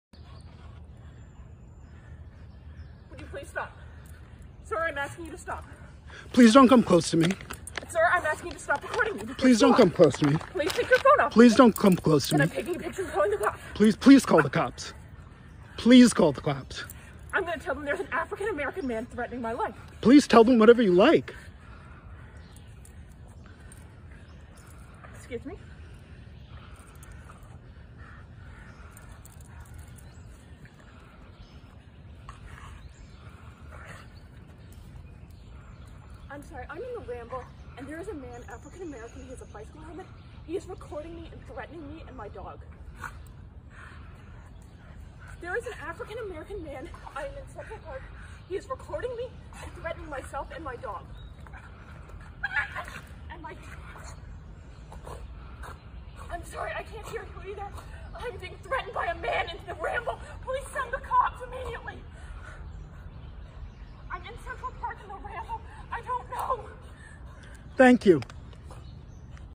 Woman calling cops on black man because he asked to put her dog on leash Central Park New York
A video has gone viral of a white woman calling the cops on a black man who simply asked her to leash her dog. The woman was in an area in New York City's Central Park known as the Ramble, where rules require that dogs be leashed.

The sister of the man who filmed the confrontation, Melody Cooper, explained that he had asked the woman to put her dog on its leash. When she refused to, he began filming.The woman in the video was identified in reports as Amy Cooper.